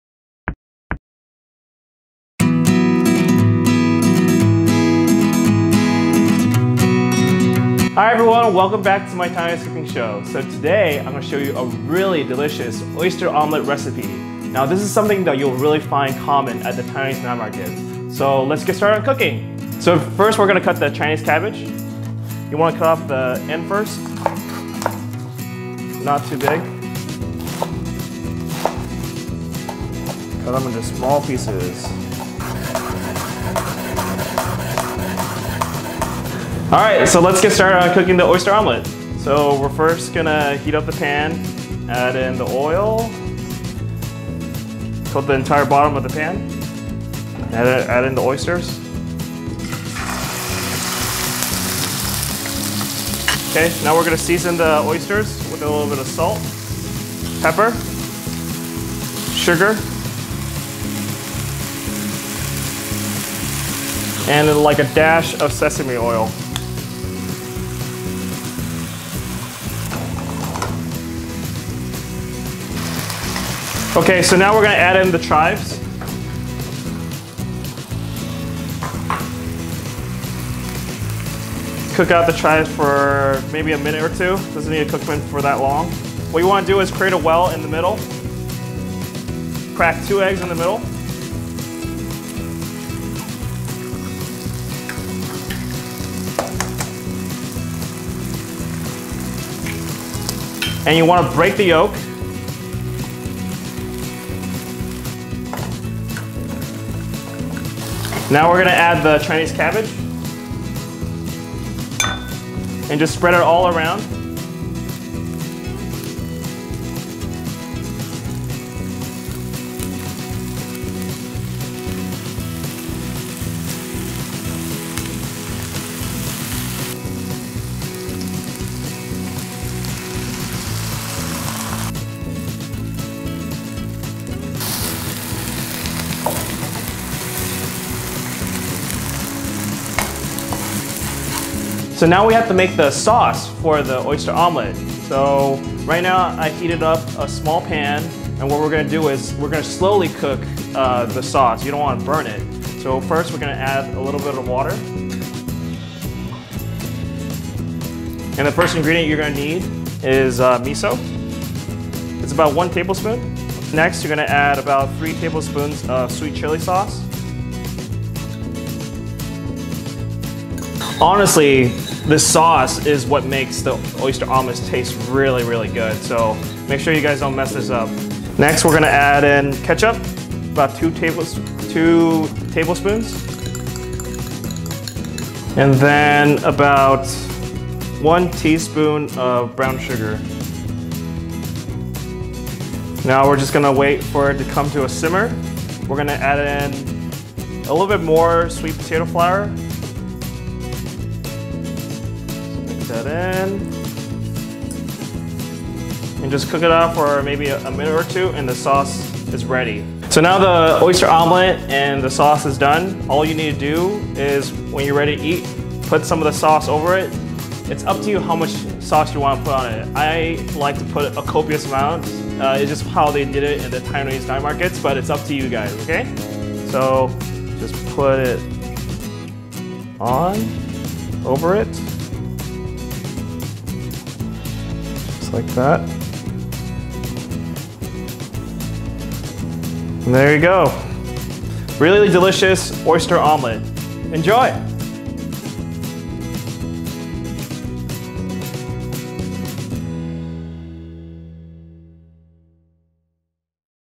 Taiwanese Oyster Omelette - 蚵仔煎 (Hézǐ jiān)
A delicious dish that you commonly find at the Taiwanese night markets. It is a dish that I always order when I have a chance. But, why order it when you can make it in a flash at the comfort of your own home?